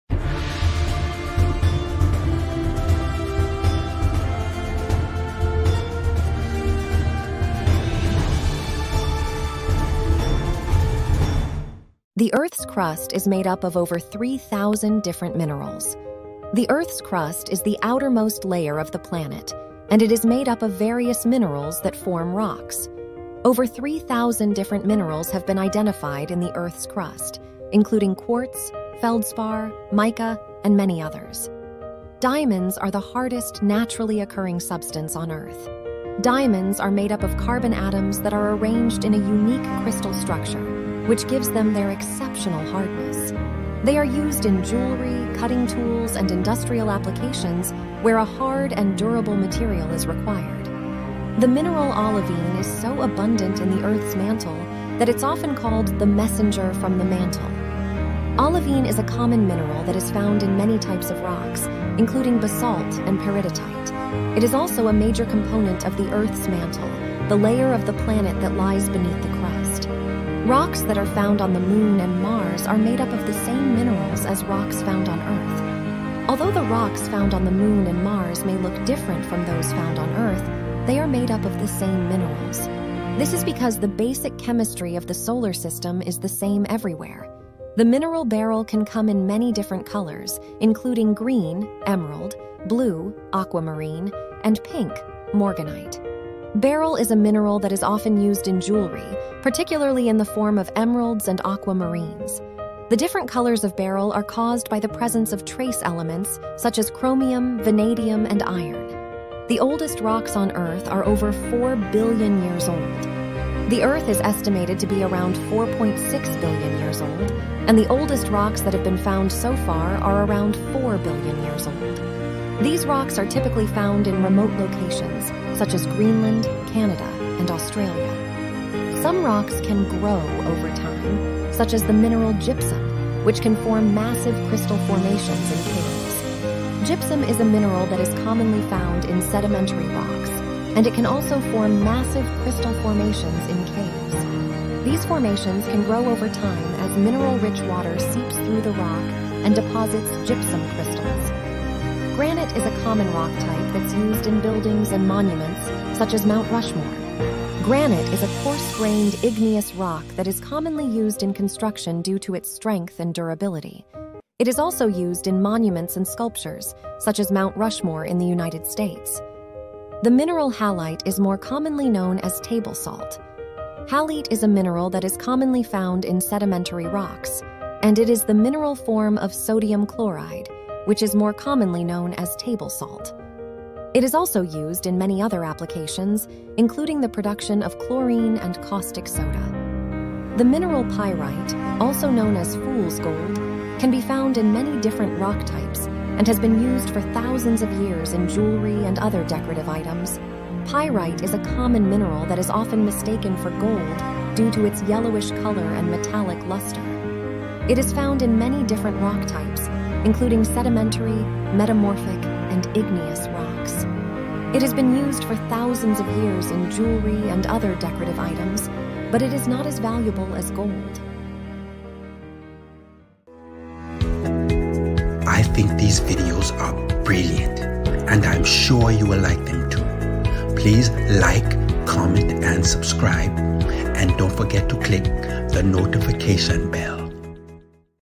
10 Fun Facts About Rocks and Minerals
The Earth's crust is made up of over 3,000 different minerals:
The Earth's crust is the outermost layer of the planet, and it is made up of various minerals that form rocks. Over 3,000 different minerals have been identified in the Earth's crust, including quartz, feldspar, mica, and many others.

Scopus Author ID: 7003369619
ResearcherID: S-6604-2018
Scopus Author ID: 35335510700